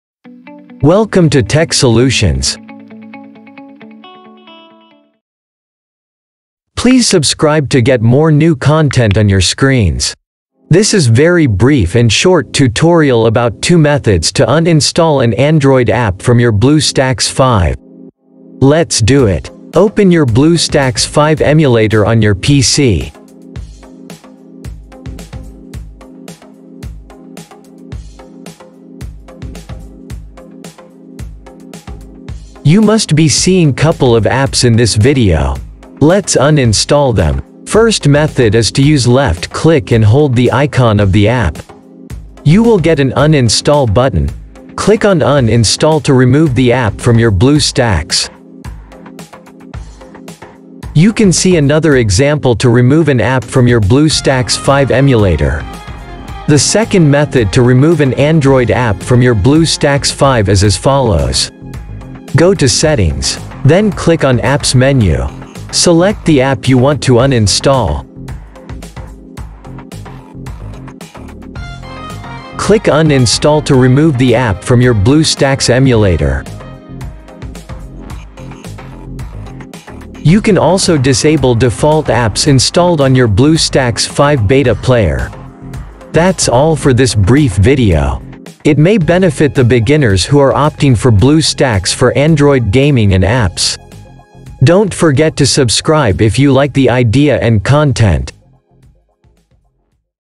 Two Ways to Uninstall an App from BlueStacks 5 2021 | Remove APK BlueStacks 5 | Android Emulator PC
Uninstall App in BlueStacks 5 2021 Remove app from BlueStacks 5 Delete an App or APK BlueStacks 5 Android Emulator in PC 2021. This very short guide is about Two ways to Remove Android Apps or APKs from your BlueStacks 5 Android Emulator on your PC. This video is a handy guide for Beginners and are new to BlueStacks 5 Android Emulator. So Watch the Video in Full and Learn Blue Stacks Easily with short guides.

PS: BlueStacks 5 is the latest android Emulator for Windows 10. You can download it from BlueStacks 5 Official Website.